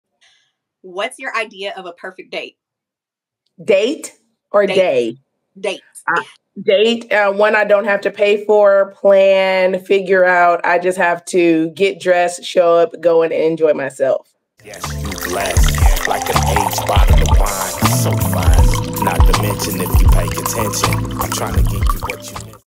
Is This The Ideal Date?
Shanica describes her idea of the perfect date. Sounds like genunine effort to me.